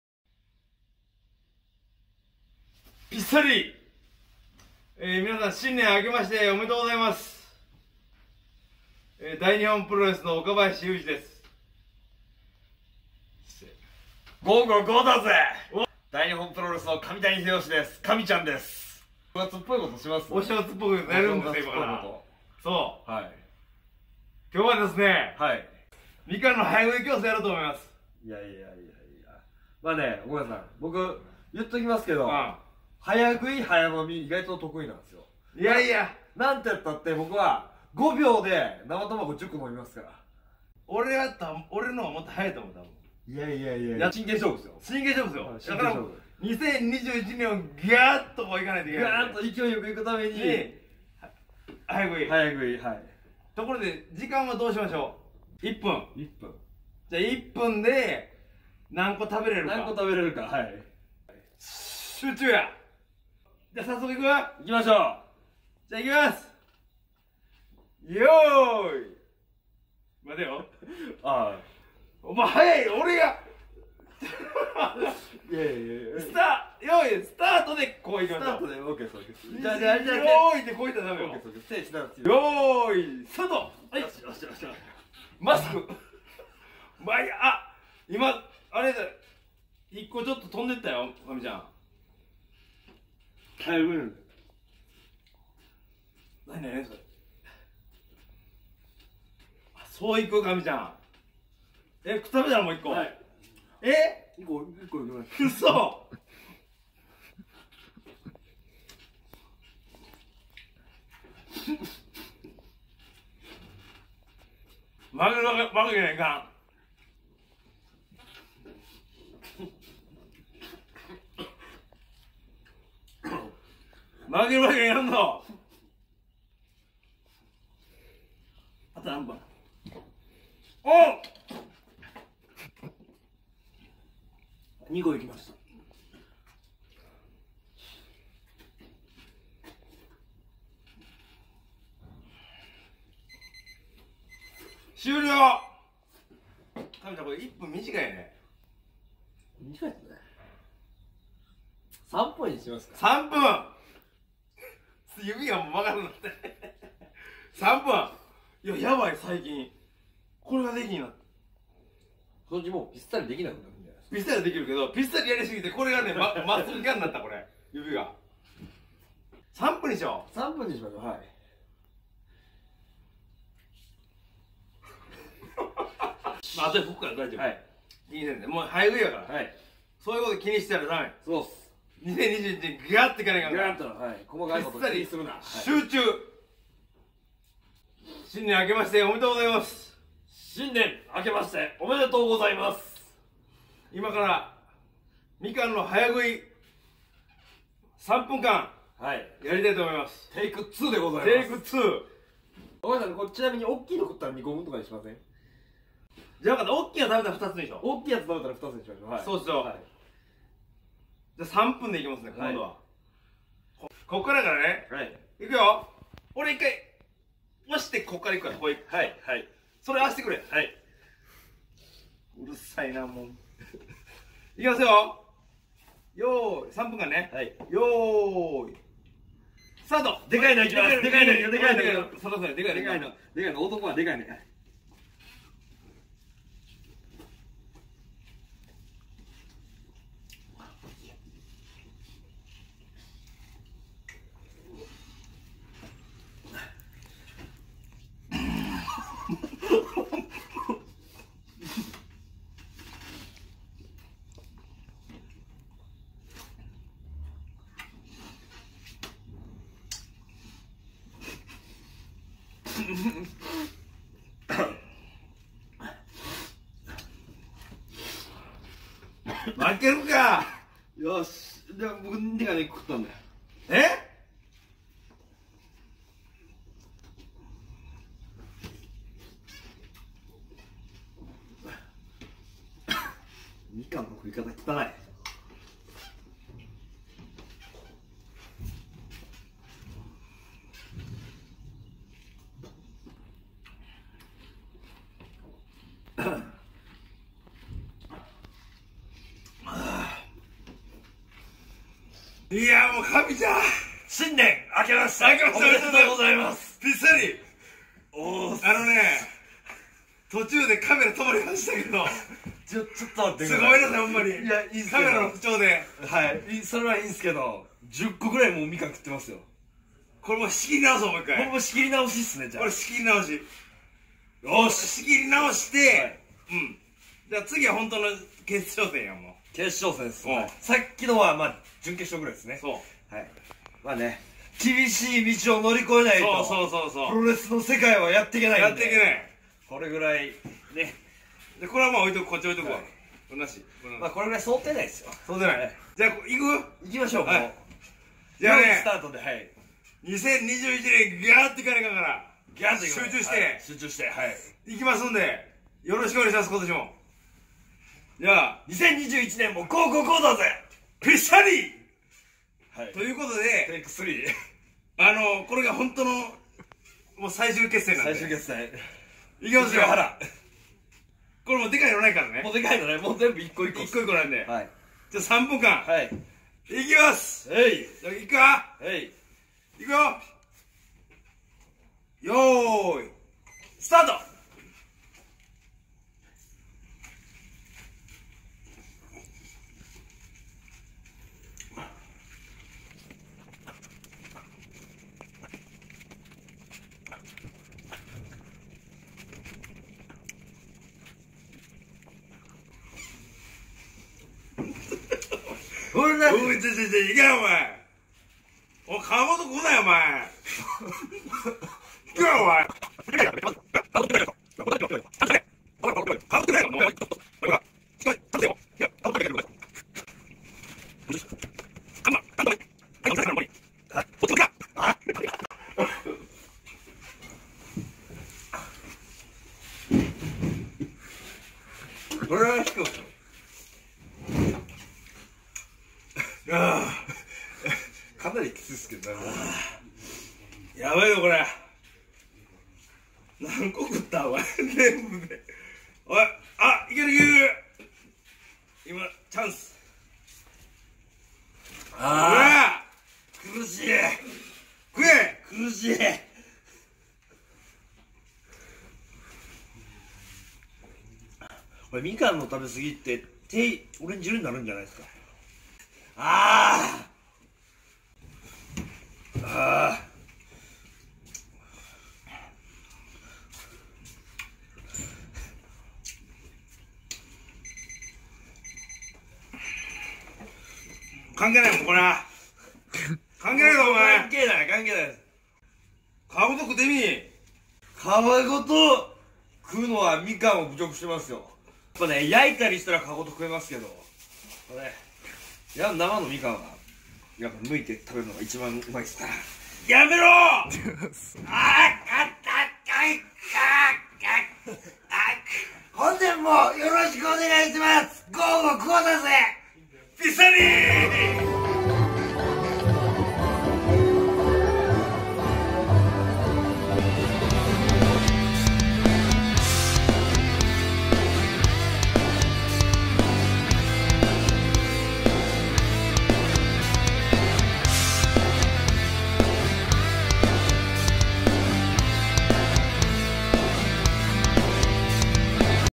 [2021年]大日本プロレス岡林裕二と神谷英慶が新年のご挨拶[新春]
皆様、新年明けましておめでとうございます🎍

本年も宜しくお願い致します🙇

久しぶりの動画投稿、新年一発目勢いをつけるためにも、是非ご視聴下さいませ🙇